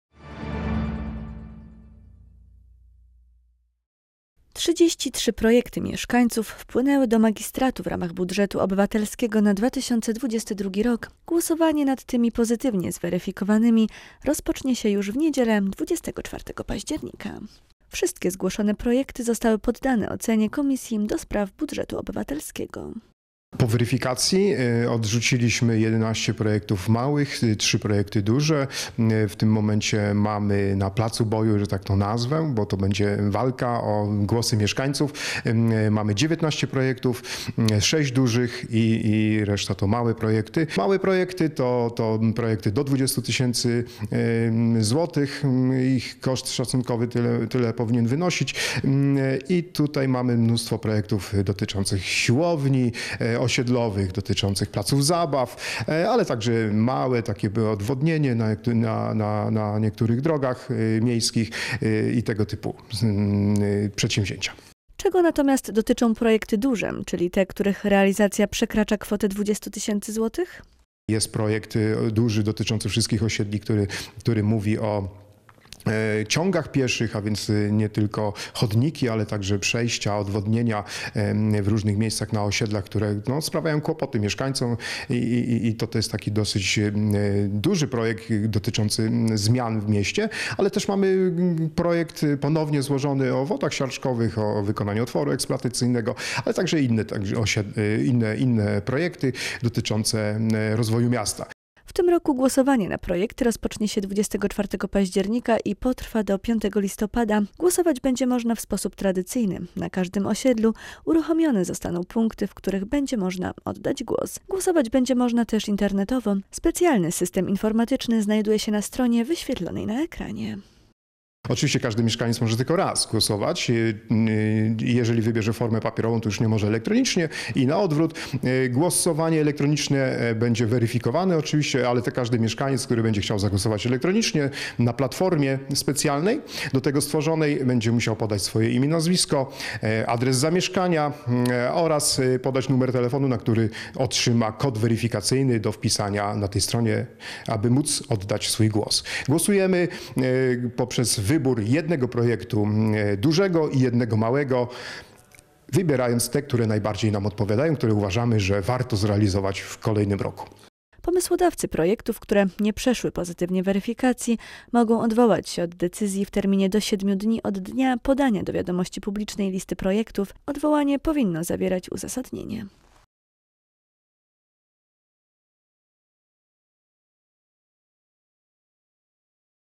2021.10.12 - PROJEKTY Z POZYTYWNĄ OPINIĄ
33 projekty mieszkańców wpłynęły do magistratu w ramach  budżetu obywatelskiego na 2022 rok. Głosowanie nad tymi pozytywnie zweryfikowanymi rozpocznie się już w niedzielę 24 października.